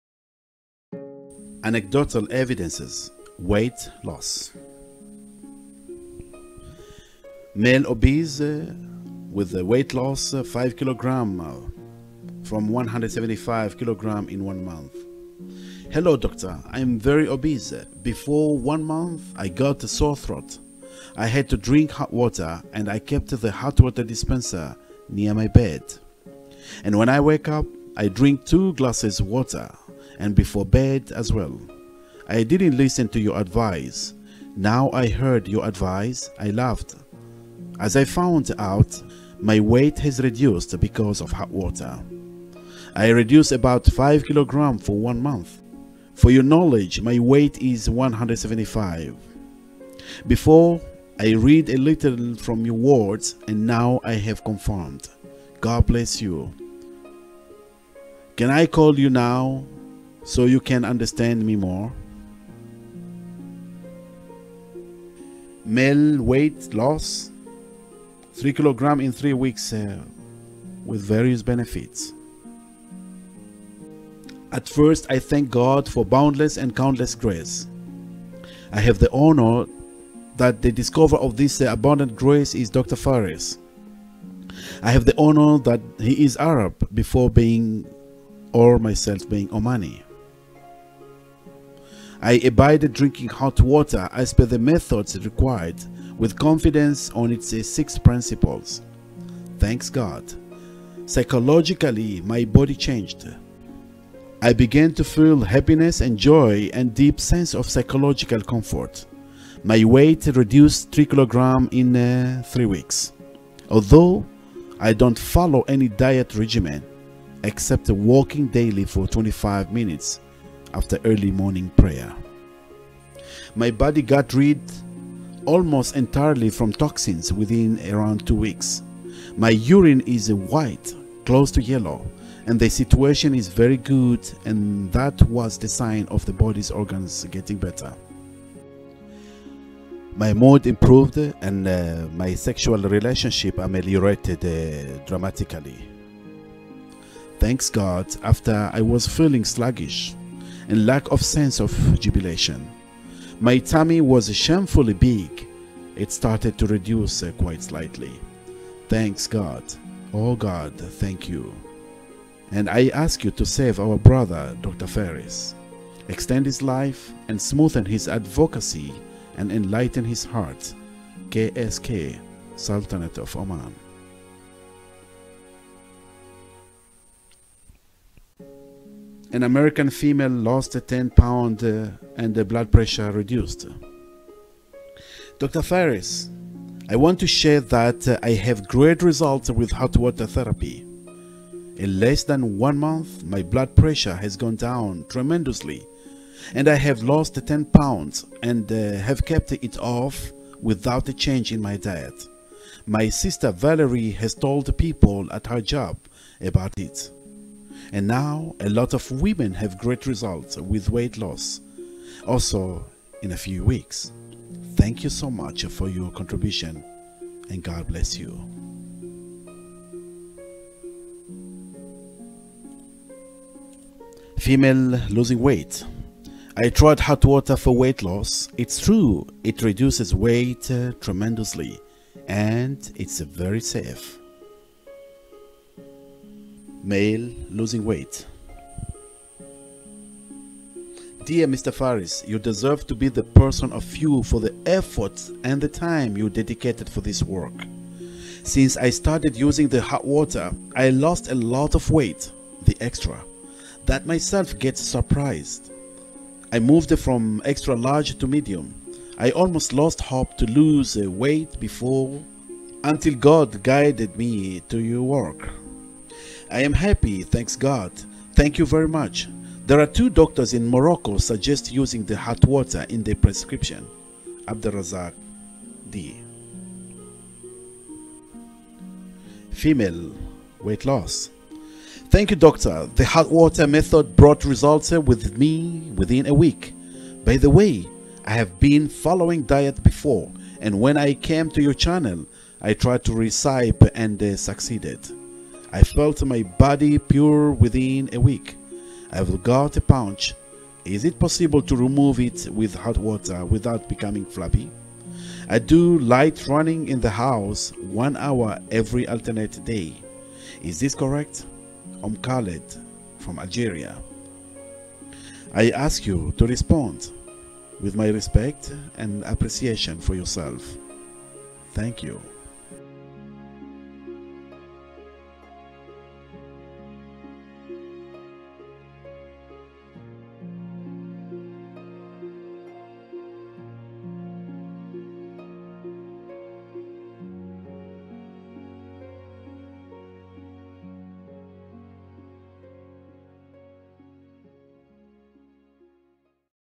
WEIGHT LOSS-1-Testimony-Haqua Revitalize® Therapy-Faris AlHajri, PhD(AM)
Weight Loss - Anecdotal Evidences - Haqua Revitalize™ Therapy - By Faris AlHajri, PhD(AM)

Hello doctor.  I am very obese. Before one month I got soar throat. I had to drink hot water, and I kept the hot water dispenser  near my bed. When I wake up, I drink 2 glasses water, and before bed as well. I didn’t listen to your advice. Now I heard your advice, I laughed, as I found out my weight has reduced because of hot water.
I reduced about 5kg for 1 month. For your knowledge my weight is 175. Before, I read a little from your words, and now I have confirmed. God bless you. Can I call you now so you can understand me more?

At first, I thank God for boundless and countless grace. I have the honor that the discoverer of this abundant grace is Dr. Faris. I have the honor that he is Arab, before being or myself being Omani.
I  abided drinking hot water as per the methods required, with confidence on its six principles. Thanks God,  psychologically, my body changed. I began to feel happiness and joy and deep sense of psychological comfort. My weight reduced 3kg in 3 weeks , although  I don’t follow any diet regimen, except walking daily for 25 minutes after early morning prayer.

My body got rid almost entirely from toxins within around 2 weeks.
My urine is white close to yellow, and the situation is very good and that was the sign of the body’s organs getting better.
My mood  improved, and my sexual  relationship  ameliorated dramatically.  Thanks God after I was feeling  sluggish,  and lack of sense of jubilation.  My tummy was  shamefully big, it started to reduce quite slightly, thanks God. Oh God thank you,  and I ask you to save our brother Dr. Faris , extend his life, and smoothen his advocacy and enlighten his heart. K.S..K – Sultanate of Oman

Dr. Faris, I want to share that I have great results with hot water therapy. In less than one month my blood pressure has gone down tremendously and I have lost 10 pounds and have kept it off without a change in my diet. My sister Valerie has told people at her job about it and now a lot of women have great results with weight loss also in a few weeks! Thank you so much for your contribution and God bless you!

I tried Hot Water for weight loss. It’s true, it reduces weight tremendously, and it’s very safe.

Dear Mr. Faris, you deserve to be the person of you for the effort and the time you dedicated for this work.
Since I started using the hot water, I lost a lot of weight (the extra) that myself gets surprised, I moved from extra large to Medium. I almost lost hope to loose weight before until God guides to your work. I am happy thanks God, Thank you very much. There are two doctors in Morocco suggest using the hot water in their prescription, …- Abderrazak D.

Thank you Dr., the hot water method brought results with me within a week. By the way, I have been following diet before, and when I came to your channel, I tried the recipe and succeeded, I felt my body pure within a week. I have got a paunch, is it possible to remove it  with hot water without becoming flabby? I do light running in the house, one  hour every alternate day. Is this correct? Om Khaled from Algeria. I ask you to respond, with my respects and appreciation for yourself. Thank you.